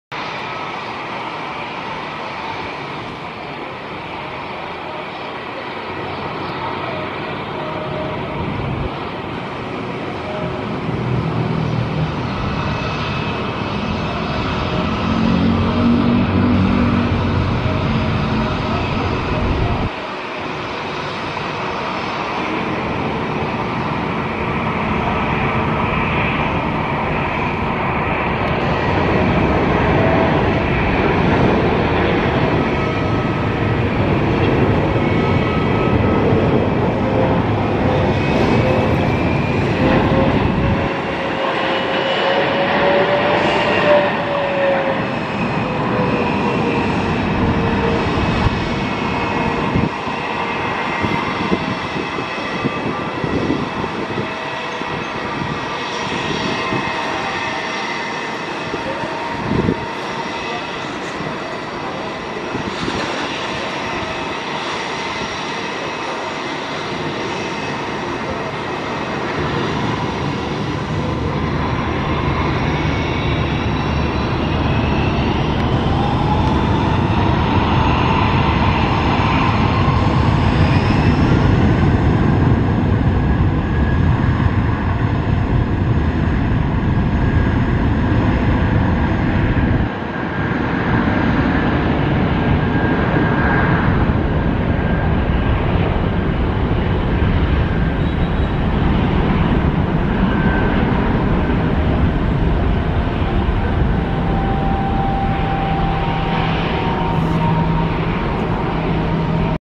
Aircraft Videos - A7-BAL - Boeing 777 3DZ(ER) Qatar Airways Landing - Plane Spotting In Airport
Plane Spotting,  landings, Take Offs, taxiing
Airbus A380, A350, A340, A330, A320, A310, A220
Boeing B777, B787, B767, B757, B737

This is the real Aircraft videos filmed on a hot day. Sorry about the bad environment and the heat haze.

Thank you from the Watching!